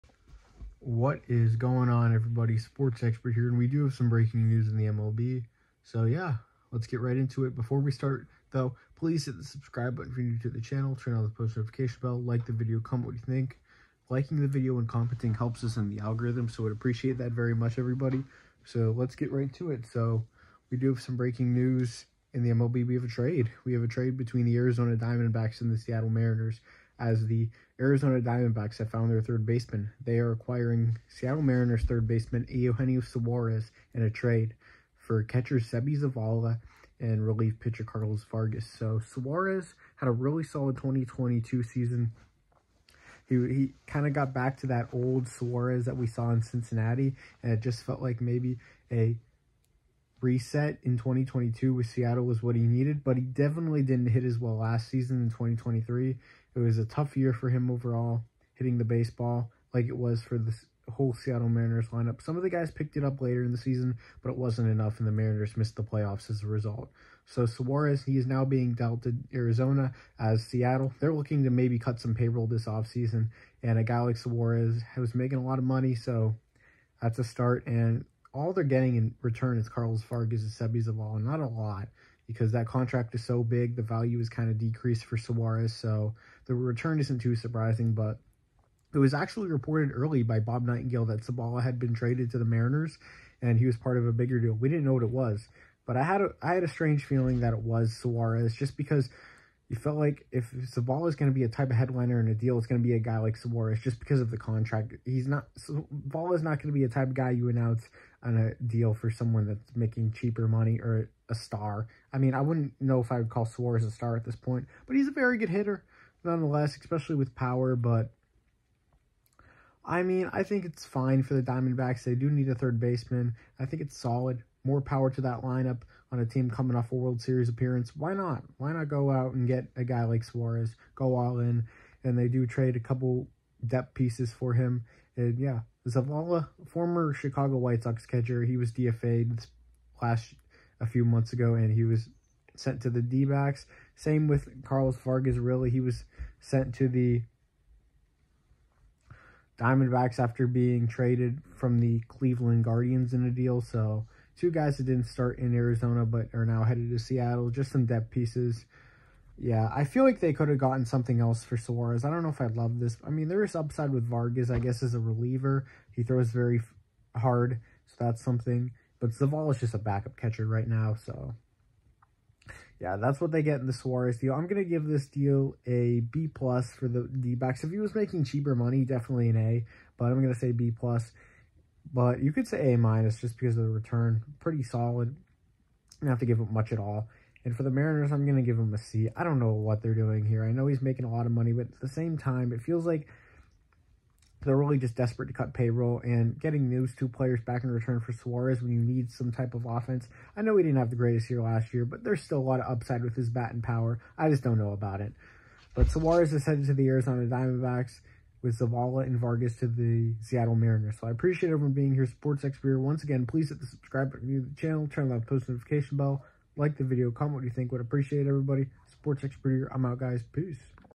Diamondbacks Acquire Eugenio Suarez In Trade With Mariners | Seby Zavala & Carlos Vargas To Seattle
Thanks for tuning in guys!!